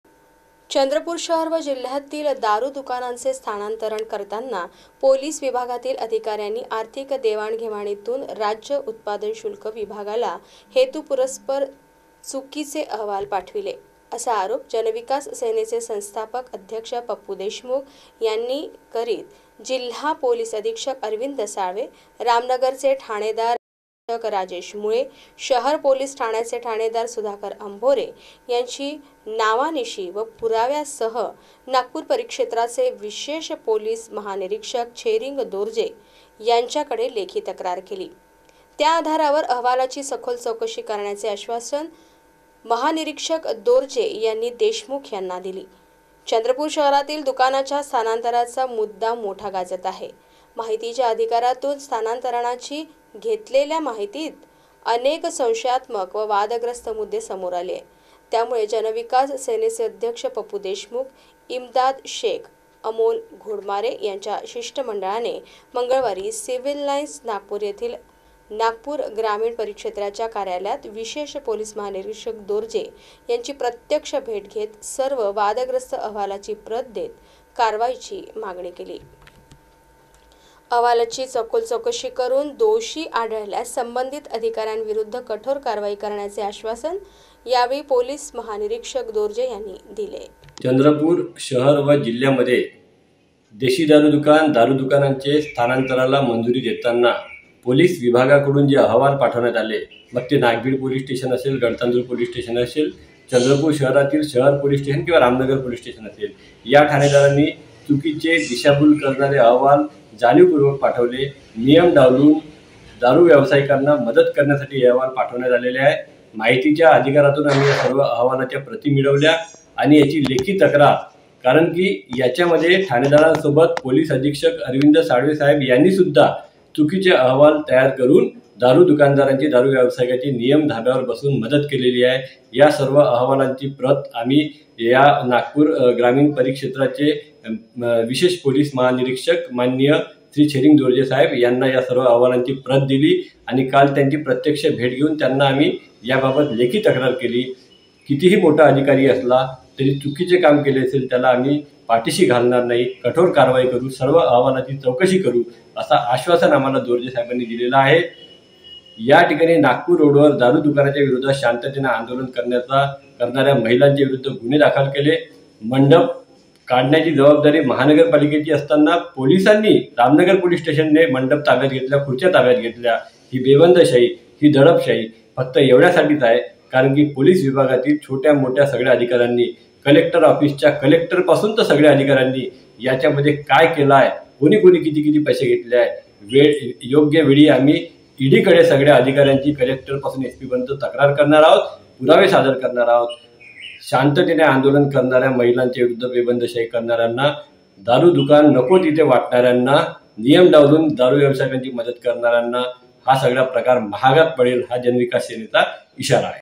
8 June 2022 SearchTv News Chandrapur जनविकास सेनेची थेट पोलिस अधीक्षकांविरुद्ध लेखी तक्रार
जनविकास सेनेची थेट पोलिस अधीक्षकांविरुद्ध लेखी तक्रार - देशी दारू दुकानांचे स्थानांतरण प्रकरण - वादग्रस्त अहवालांची चौकशी करण्याचे पोलीस महानिरीक्षकांचे आश्वासन